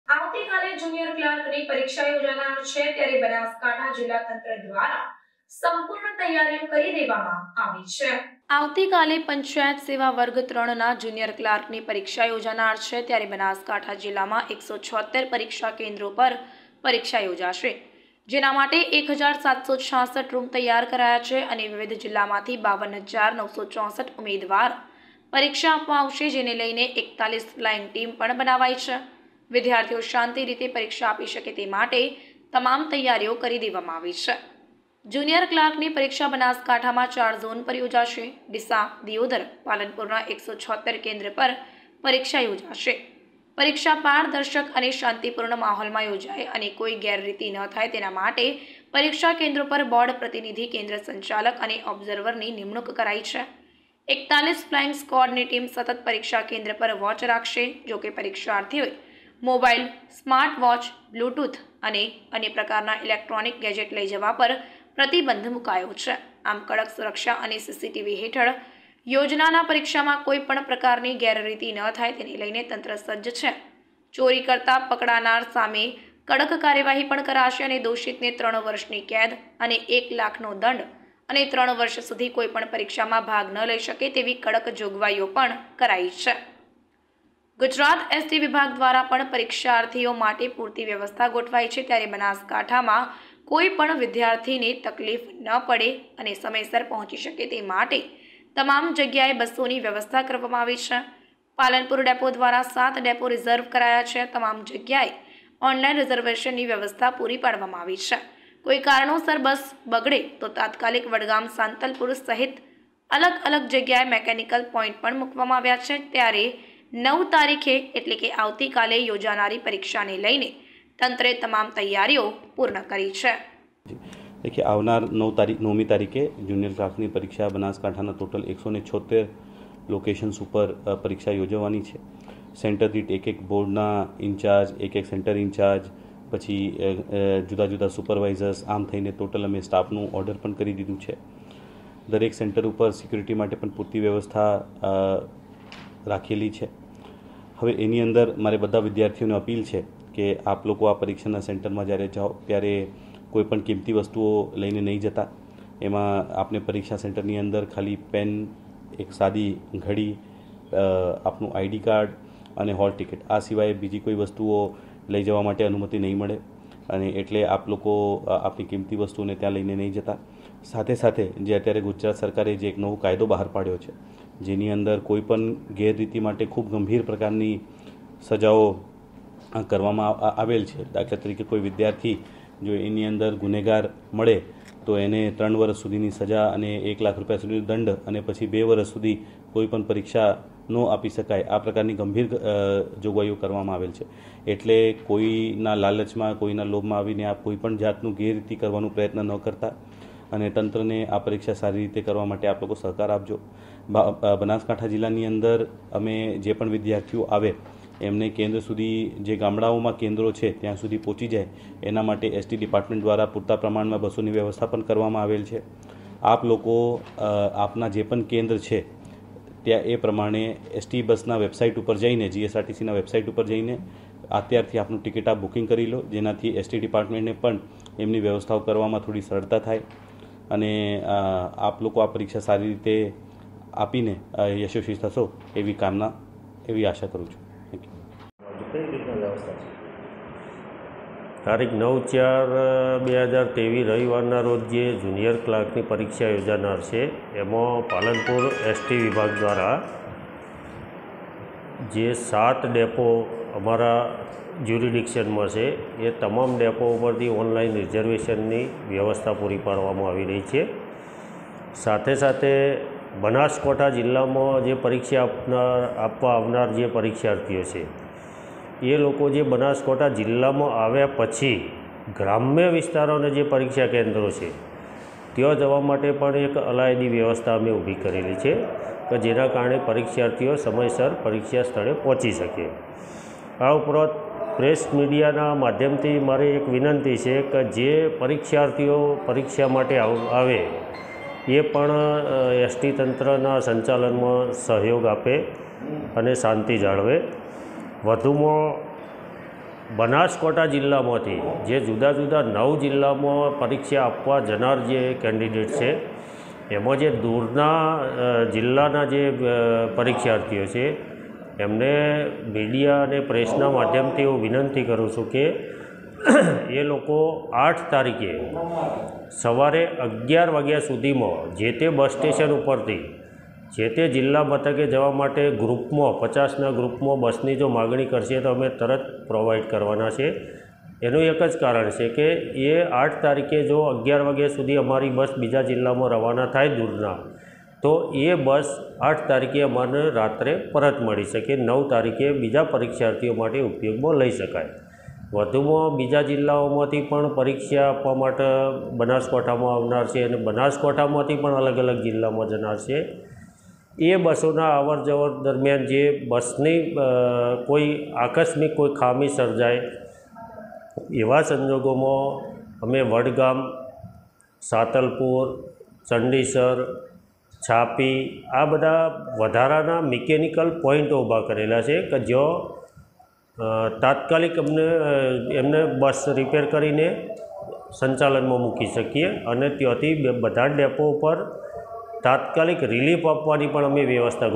જુનિયર ક્લાર્કની પરીક્ષા યોજાનાર છે ત્યારે બનાસકાંઠા જિલ્લા તંત્ર દ્વારા સંપૂર્ણ તૈયારીઓ
આવતીકાલે જુનિયર ક્લાર્કની પરીક્ષા યોજાનાર છે ત્યારે બનાસકાંઠા જિલ્લા તંત્ર દ્વારા સંપૂર્ણ તૈયારીઓ કરી દેવામાં આવી છે.

બનાસકાંઠા..

પરીક્ષા તૈયારી
.9 એપ્રિલ પંચાયત સેવા વર્ગ ત્રણના જુનિયર ક્લાર્ક ની પરીક્ષા યોજનાર છે બનાસકાંઠા જિલ્લામાં 176 પરીક્ષા કેન્દ્રો પર પરીક્ષા યોજાશે જેના માટે 1766 રૂમ તૈયાર કરાયા છે અને વિવિધ જિલ્લામાંથી 52,964 ઉમેદવાર પરીક્ષા આપવા આવશે જેને લઈને 41 ફ્લાઈંગ ટીમ પણ બનાવાઈ છે .વિદ્યાર્થીઓ શાંતિ રીતે પરીક્ષા આપી શકે તે માટે તૈયારીઓ કરાઈ છે.
જુનિયર ક્લાર્ક ની પરીક્ષા બનાસકાંઠામાં ચાર ઝોન પર યોજાશે ડીસા દિયોદર પાલનપુર  ના 176 કેન્દ્ર પર પરીક્ષા યોજાશે પરીક્ષા પારદર્શક અને શાંતિપૂર્ણ માહોલમાં યોજાય અને કોઈ  ગેરરીતી ન થાય તેના માટે પરીક્ષા કેન્દ્રો પર બોર્ડ પ્રતીનીધી કેન્દ્ર સંચાલક અને ઓબ્ઝર્વની નિમણૂક કરાય છે 41 ફ્લાઈંગ સ્કવોડ ની ટીમ સતત પરીક્ષા કેન્દ્ર પર વોચ રાખશે જોકે પરીક્ષાર્થીઓએ મોબાઈલ સ્માર્ટ વોચ બ્લુટુથ અને અન્ય પ્રકારના ઇલેક્ટ્રોનિક ગેજેટ લઈ જવા પર પ્રતિબંધ મુકાયો છે આમ કડક સુરક્ષા અને સીસીટી હેઠળ યોજનાના પરીક્ષામાં કોઈપણ પ્રકારની ગેરરીતિ ન થાય તેને લઈને તંત્ર સજ્જ છે.ચોરી કરતા પકડાનાર સામે કડક કાર્યવાહી પણ કરાશે અને દોષિતને ત્રણ વર્ષની કેદ અને એક લાખનો દંડ અને ત્રણ વર્ષ સુધી કોઈ પણ પરીક્ષામાં ભાગ ન લઈ શકે તેવી કડક જોગવાઈઓ પણ કરાઈ છે

બાઈટ.. વરૂણ બરનવાલ.. કલેકટર બનાસકાંઠા

વી ઓ..ગુજરાત એસટી વિભાગ દ્વારા પણ પરિક્ષાથીઓ માટે પૂરતી વ્યવસ્થા ગોઠવાઈ છે.ત્યારે બનાસકાંઠા માં.કોઈ પણ વિધાયાર્થી ને તકલીફ નાં પડે અને સમયસર પહોંચી શકે તે માટે તમામ જગ્યા એ બસો ની વ્યવસ્થા કરવામાં આવી છે. પાલનપુર ડેપો દ્વારા સાત ડેપો રીજરવ કરાયા છે.તમામ જગ્યા એ ઓનલાઇન રીજરવેશન ની વ્યવસ્થા પૂરી પાડવામાં આવી છે.કોઈ કારણોસર બસ બગડે તો તાત્કાલિક વડગામ સાંતલપુર સહિત અલગ અલગ જગ્યા એ  મેકનીકલ પોઇન્ટ પણ મૂકવામાં આવ્યા છે.ત્યારે 9 તારીખે યોજાનારી પરીક્ષા ને લઇ તંત્ર એ તમામ તૈયારીઓ પૂર્ણ કરી છે.

બાઈટ.. વિનું ભાઈ પટેલ..એસ ટી અધિકારી

અહેવાલ : દિપક સોલંકી પાલનપુર